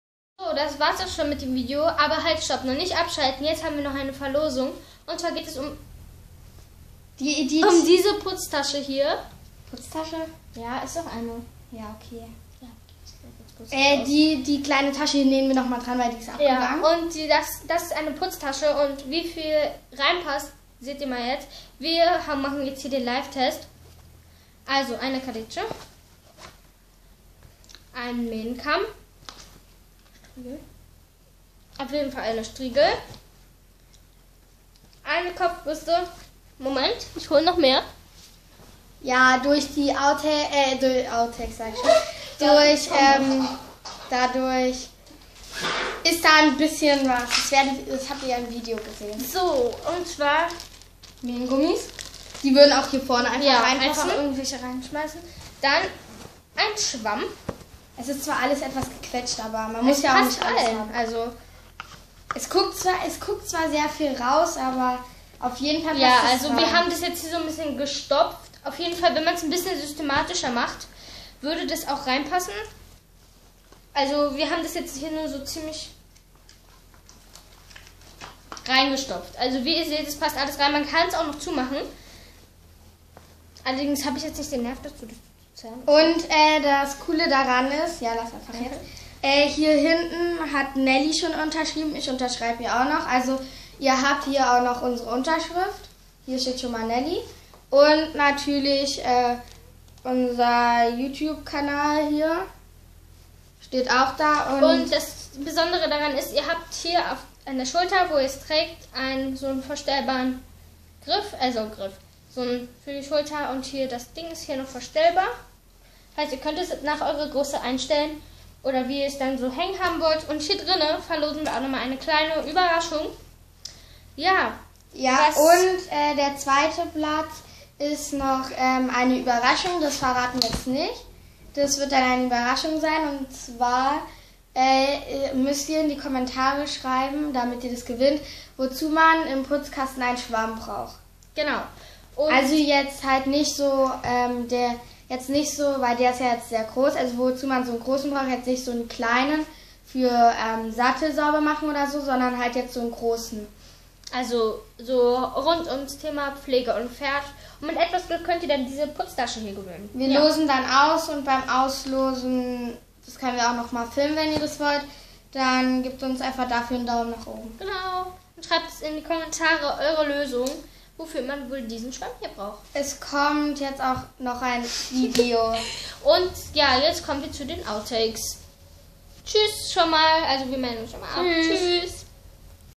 Gewinnspiel | INFOBOX LESEN
Hallo Leute
hier ist der 3. Teil sozusagen von dem 5 Arten Video hier ist ein kleines Gewinnspiel.
Wir hoffe das ihr mitmacht.
Liebe Grüße Lena und Nelly 💙 💚 💛 💜 🐴